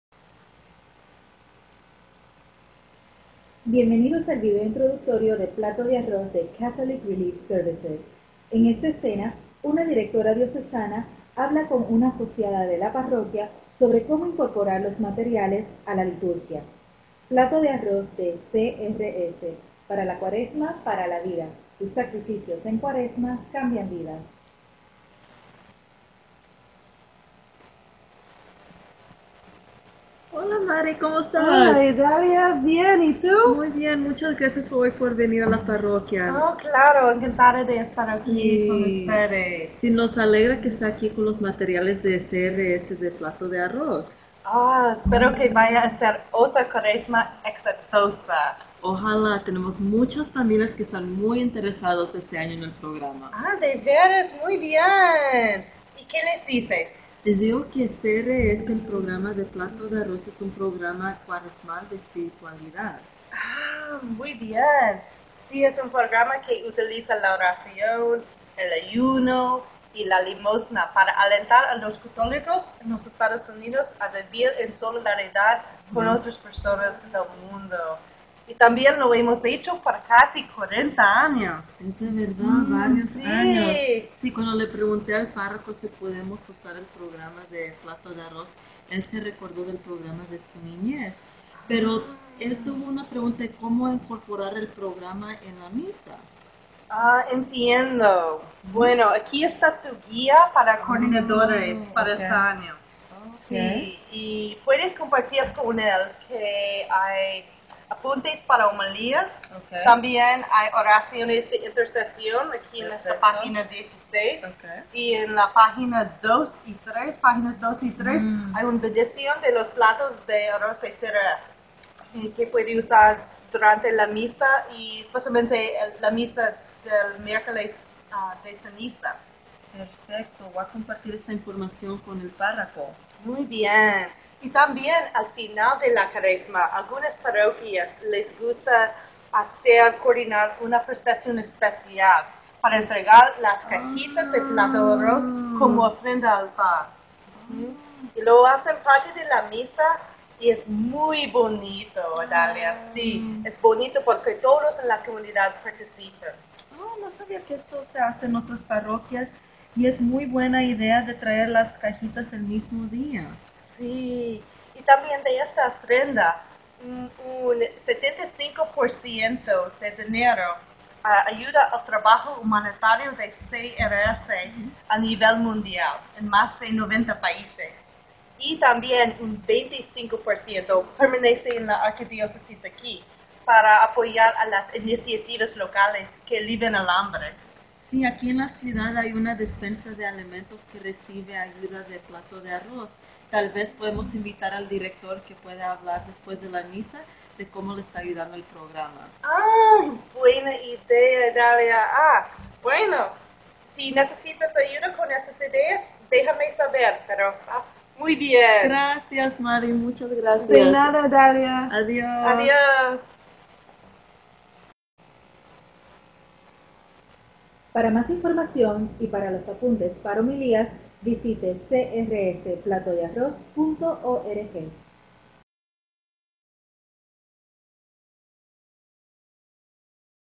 Plato de Arroz en la parroquia
Bienvenidos al video de presentación de Plato de Arroz de CRS. En esta escena una directora diocesana habla con una asociada de la parroquia sobre cómo incorporar los materiales a la liturgia.